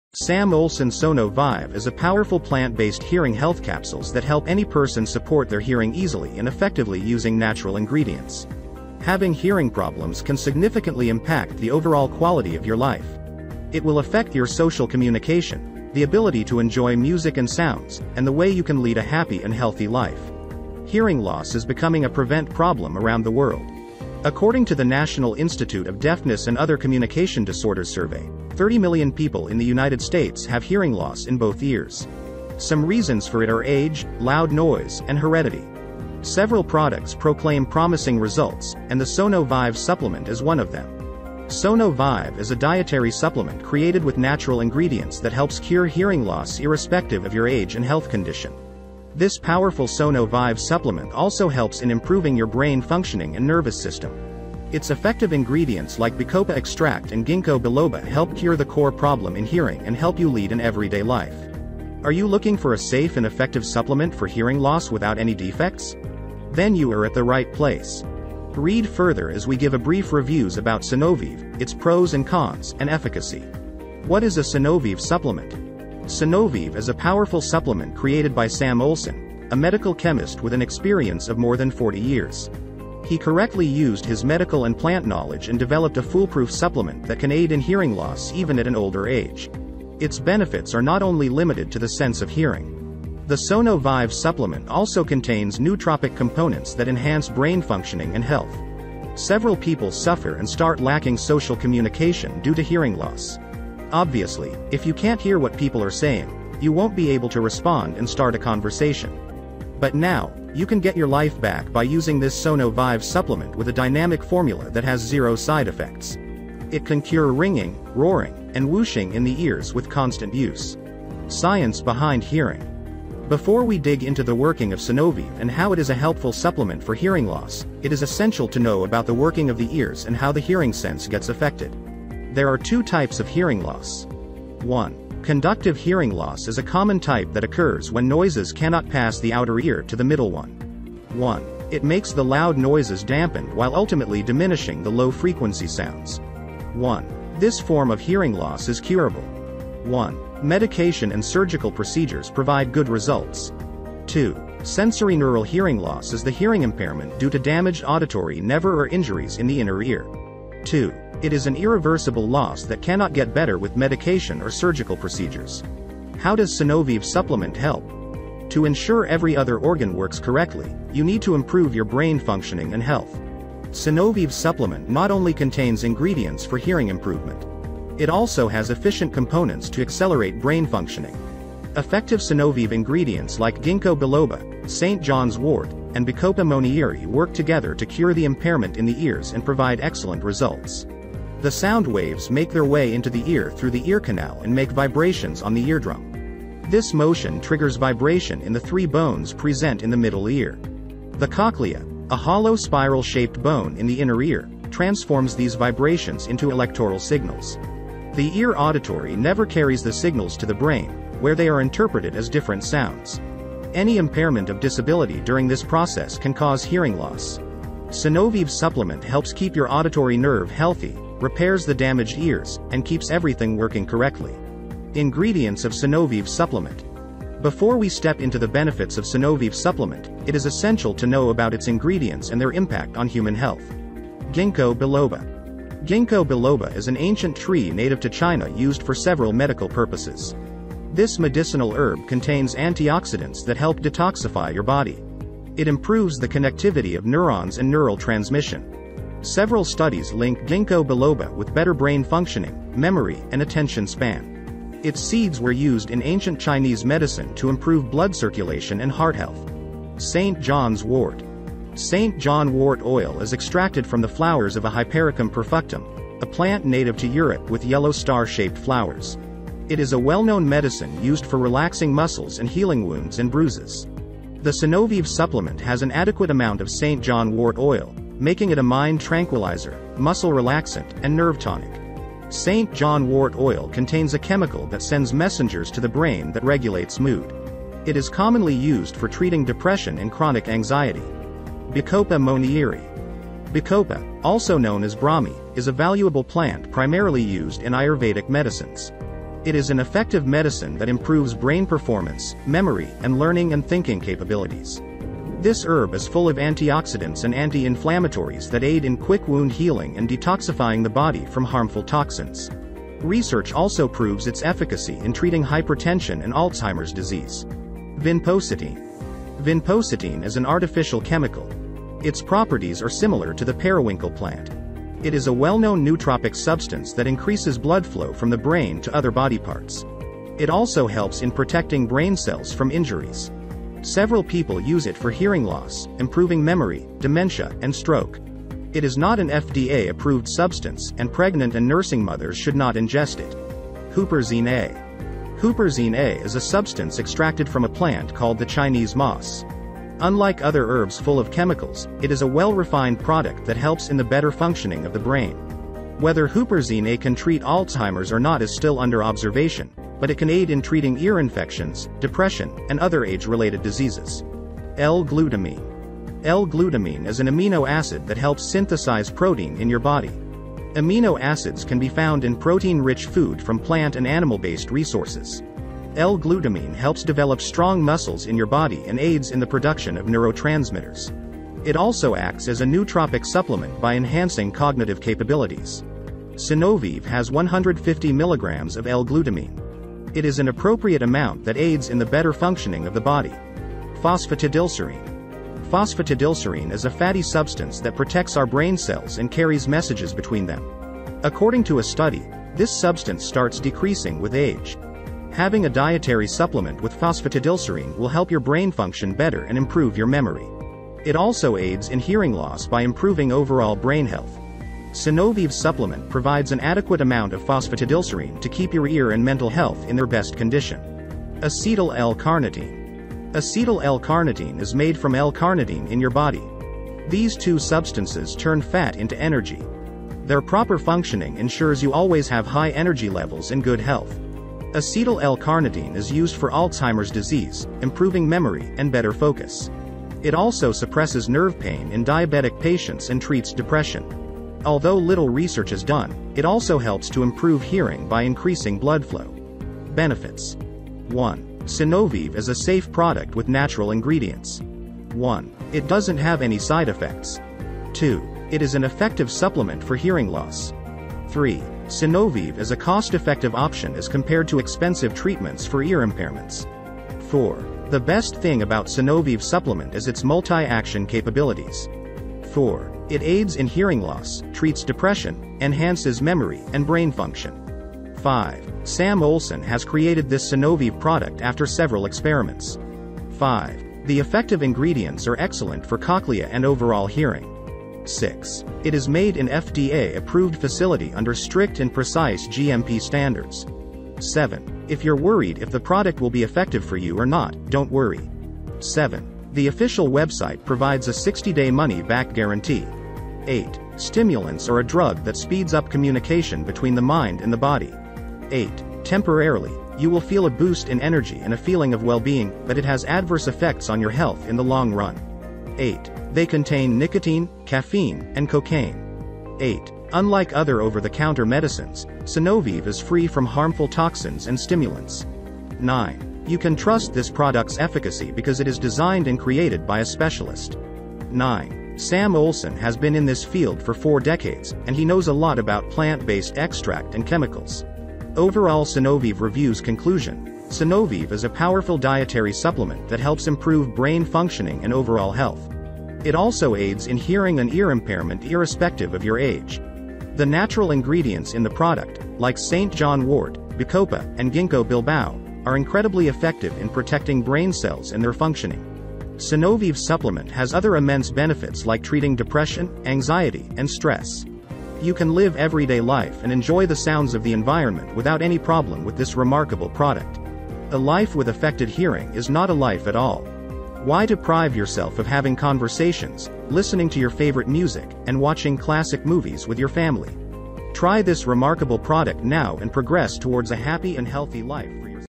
SONOVIVE Review 2022 - The Whole TRUTH About The SONOVIVE! Sonovive Works
SonoVive is an effective advanced hearing health formula that promotes hearing health and supports brain function. This supplement works effectively for anyone at any age without causing any side effects.